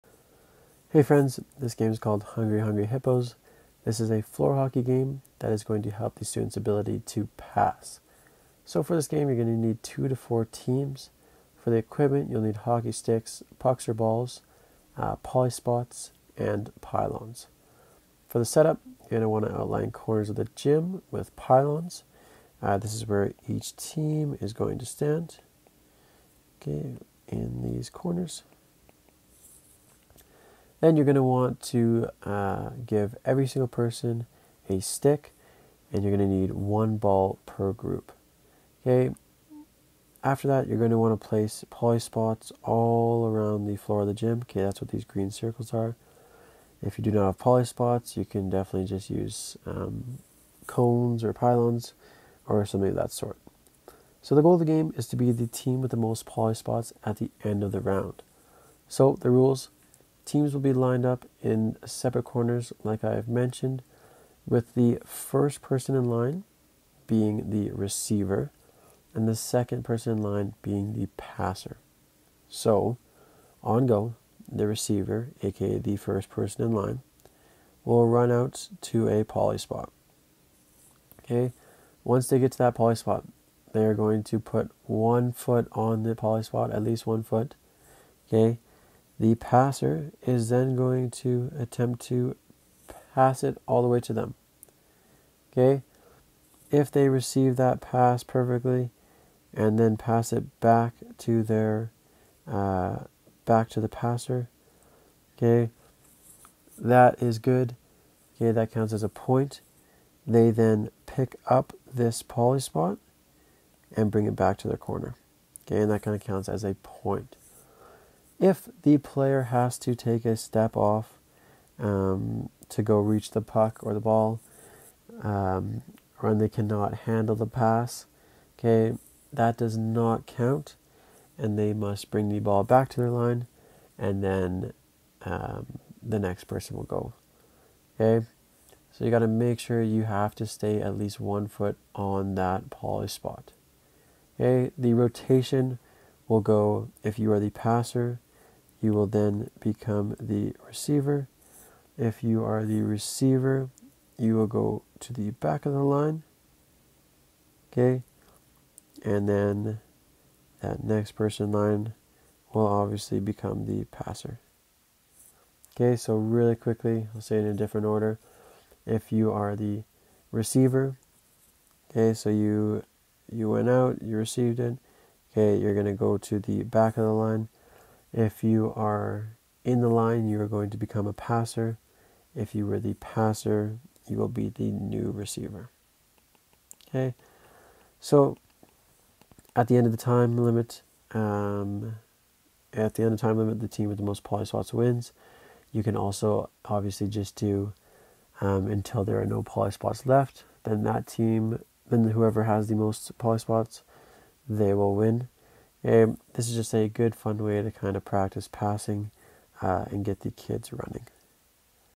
Hungry Hungry Hippos (PE Floor Hockey)
Hey friends,

Floor hockey PE game focused on passing.